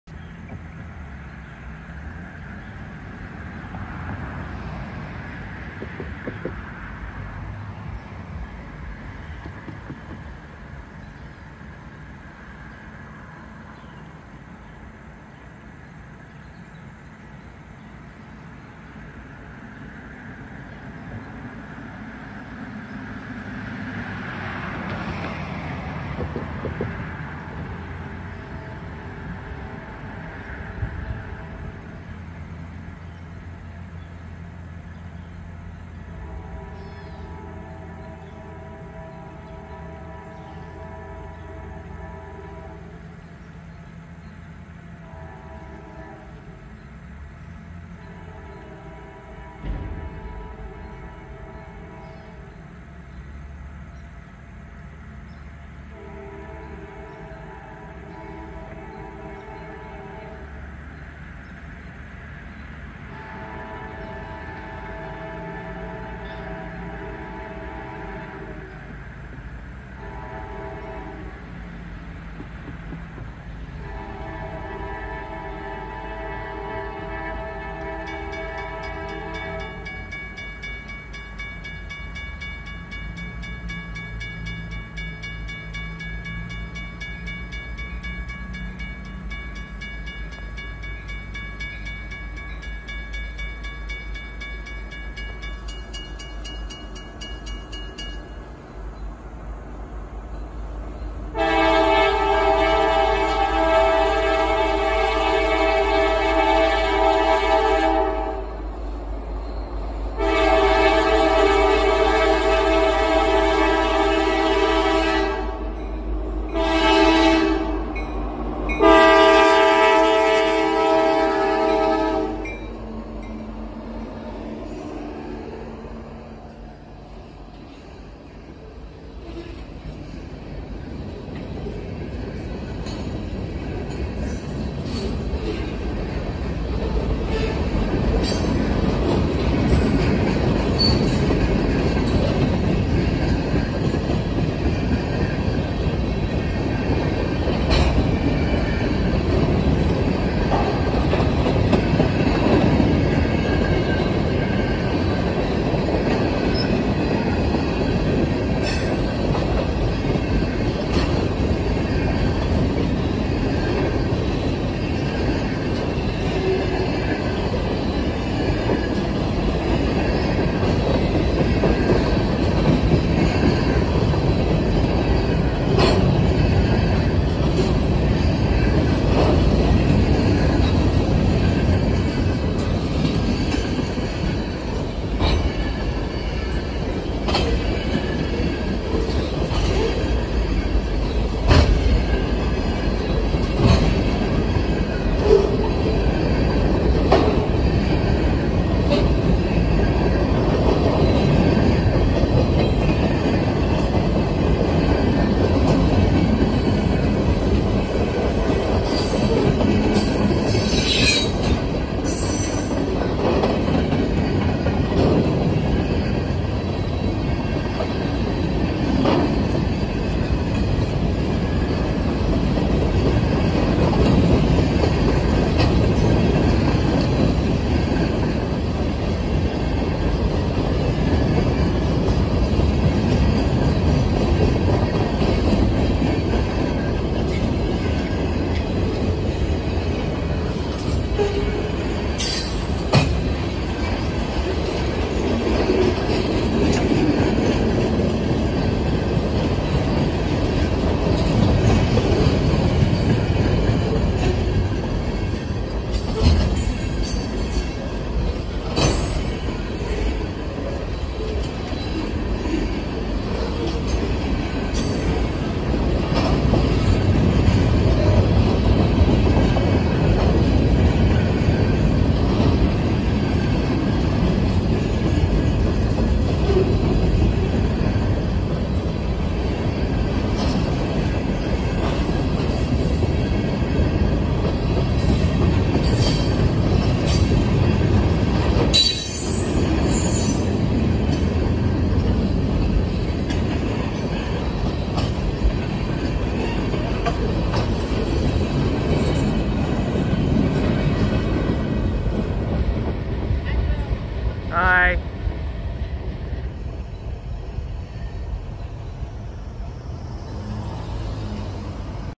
Norfolk Southern H95 - 8/3/22
Norfolk Southern local freight train H95 is seen crossing Chestnut Street on the York Secondary with a Nathan K5LA horn in Mount Wolf, Pennsylvania at 8:52 AM on Wednesday, August 3, 2022. The engines in this video are: NS 6340 (SD40E) and NS 6943 (SD60E). Enjoy!